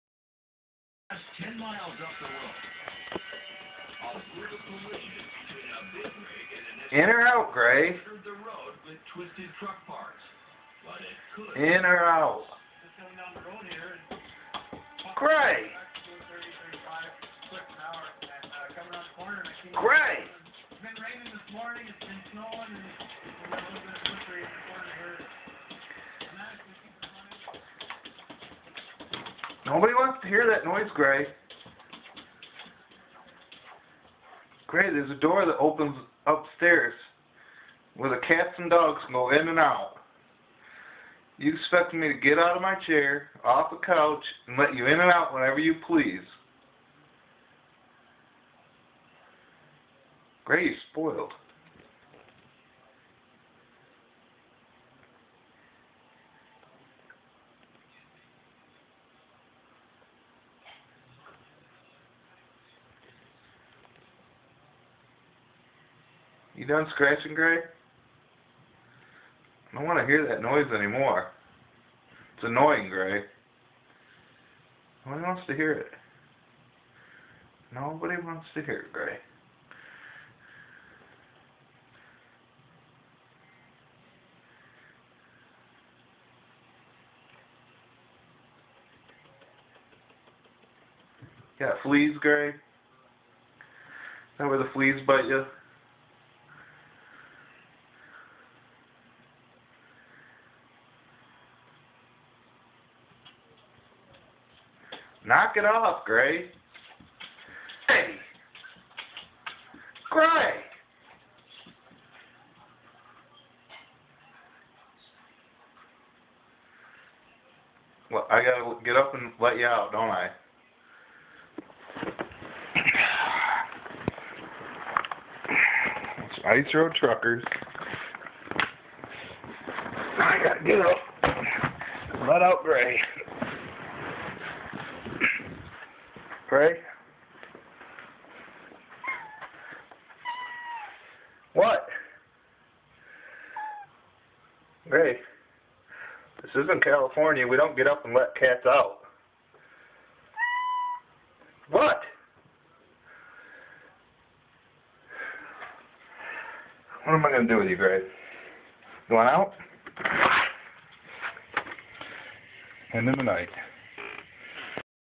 I think cat (Missy Grey) wants out?
Now she wants out... IN OR OUT Grey!  There is a pet door upstairs that she could use, but OH NO, she makes me get up to let her in/out.